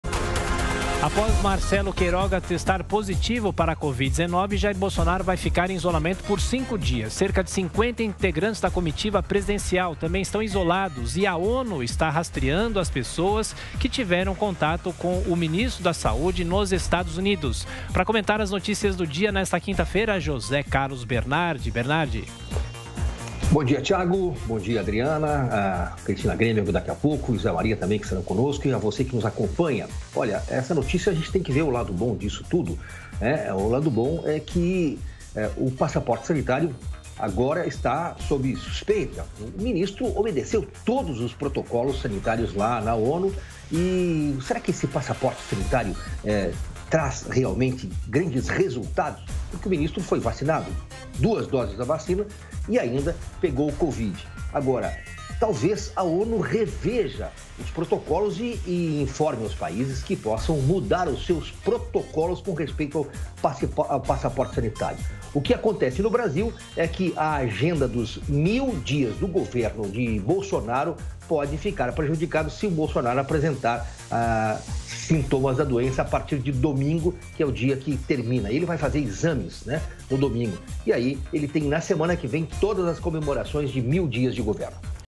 Bernardi: Será que o passaporte sanitário traz grandes resultados?
Após Marcelo Queiroga testar positivo para a COVID-19, Jair Bolsonaro ficará em isolamento por cinco dias. 50 integrantes da comitiva presidencial também estão isolados e a ONU está rastreando as pessoas que tiveram contato com o Ministro da Saúde nos Estados Unidos. José Carlos Bernardi comenta.